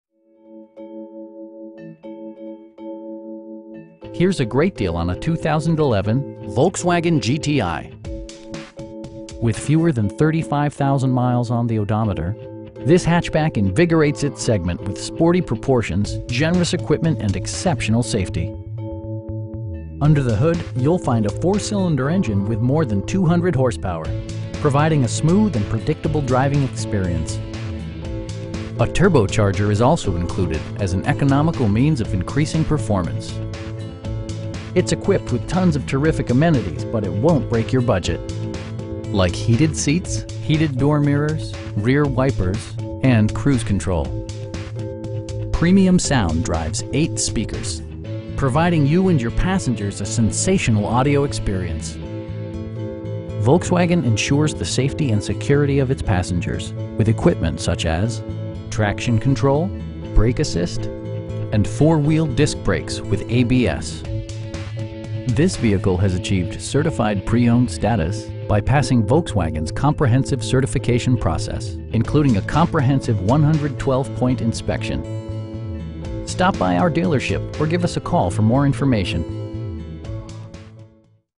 2011 Volkswagen GTI w/Sunroof in Sterling, VA 20166
Lindsay Volkswagen of Dulles
22455 Cedar Green Road

Here's a great deal on a 2011 Volkswagen GTI. With fewer than 35,000 miles on the odometer, this hatchback invigorates its segment with sporty proportions, generous equipment and exceptional safety! Under the hood you'll find a 4 cylinder engine with more than 200 horsepower, providing a smooth and predictable driving experience. A turbocharger is also included as an economical means of increasing performance. It's equipped with tons of terrific amenities, but it won't break your budget. Like heated seats, front and rear reading lights, air conditioning, heated door mirrors, remote keyless entry, an overhead console, rear wipers, and cruise control. Storage solutions are integrated throughout the interior, demonstrating thoughtful attention to detail. Premium sound drives 8 speakers, providing you and your passengers a sensational audio experience. Volkswagen ensures the safety and security of its passengers with equipment such as: head curtain airbags, front SIDE impact airbags, traction control, brake assist, anti-whiplash front head restraints, ignition disabling, and 4 wheel disc brakes with ABS. With electronic stability control supplementing mechanical systems, you'll maintain precise command of the roadway. This vehicle has achieved Certified Pre-Owned status, by passing Volkswagen's comprehensive certification process, including a comprehensive 112 point inspection! We pride ourselves in the quality that we offer on all of our vehicles. Stop by our dealership or give us a call for more information.